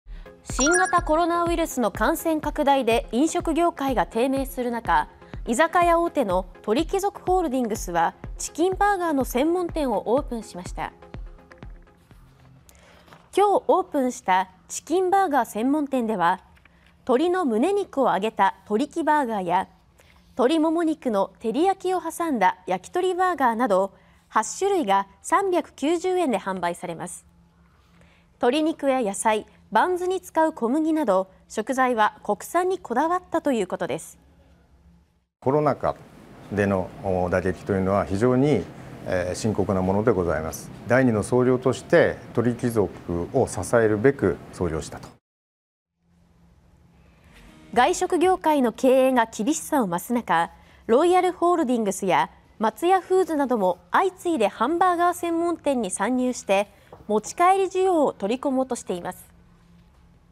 「焼鳥バーガー」 居酒屋大手がコロナ対策で専門店(2021年8月23日)
新型コロナウイルスの感染拡大で飲食業界が低迷するなか、居酒屋大手「鳥貴族ホールディングス」はチキンバーガー専門店をオープンしました。

　23日にオープンしたチキンバーガー専門店では、鶏のむね肉を揚げた「トリキバーガー」や鶏もも肉の照り焼きを挟んだ「焼鳥バーガー」など8種類が390円で販売されます。

　鶏肉や野菜、バンズに使う小麦など食材は国産にこだわったということです。

　TORIKI　BURGER・高田哲也代表取締役社長：「コロナ禍での打撃というのは非常に深刻。第二の創業として『鳥貴族』を支えるべく創業した」

　外食業界の経営が厳しさを増すなか、ロイヤルホールディングスや松屋フーズなども相次いでハンバーガー専門店に参入して持ち帰り需要を取り込もうとしています。